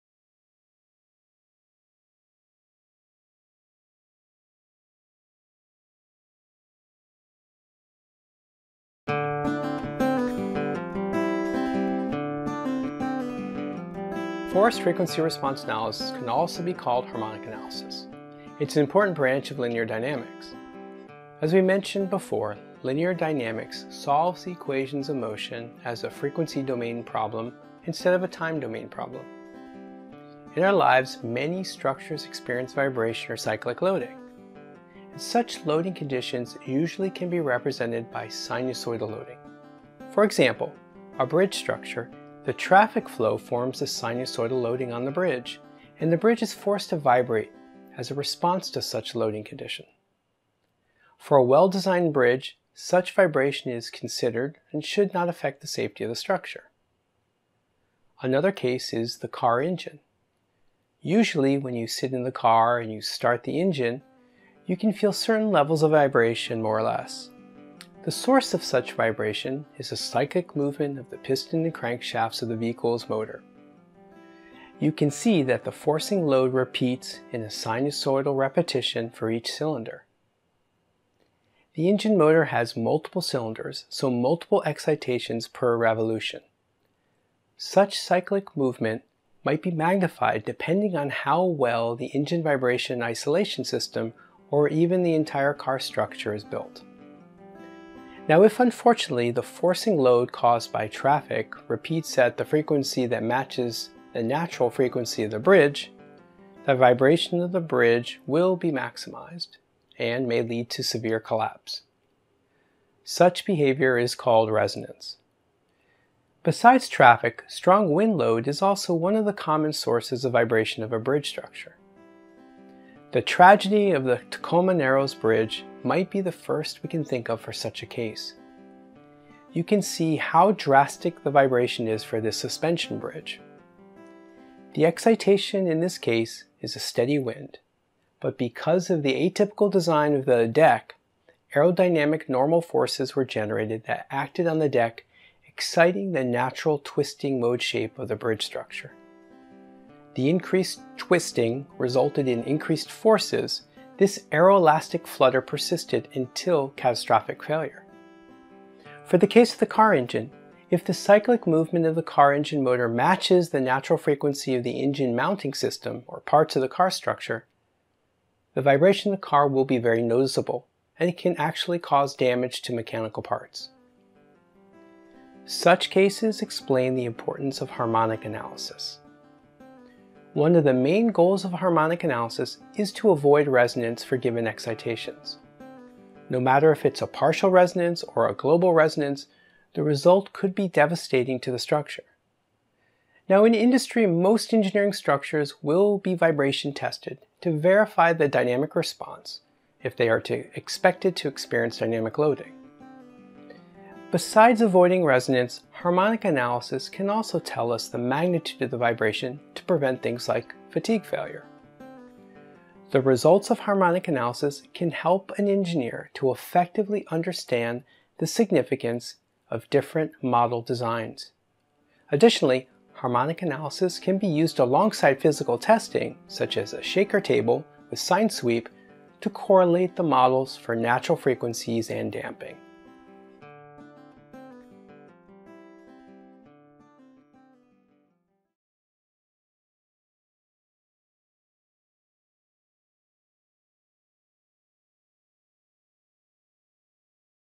Intro Harmonic Analysis — Lesson 1
This video lesson introduces harmonic analysis as a branch of linear dynamics, which involves solving equations of motion in the frequency domain instead of the time domain. Its goal is to avoid potentially destructive resonance vibration in structures for given excitations. This course is part of the Ansys Innovation Course: Harmonic Analysis of Structures. To access this and all of our free, online courses — featuring additional videos, quizzes and handouts — visit Ansys Innovation Courses